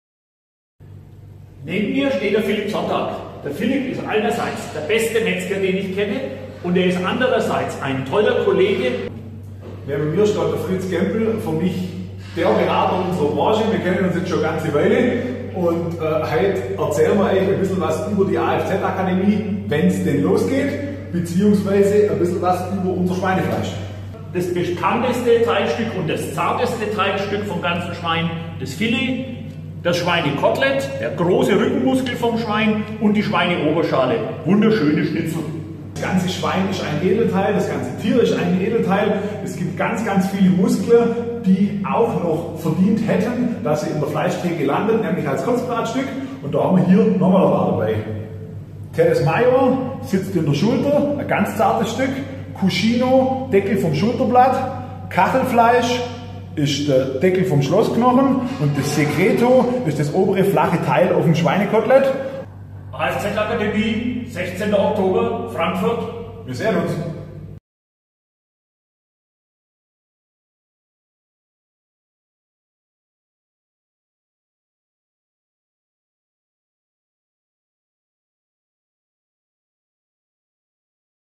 afz video neu Full HD 1080p
Besuch bei Fleischsommelier Philipp Sontag in Kisslegg.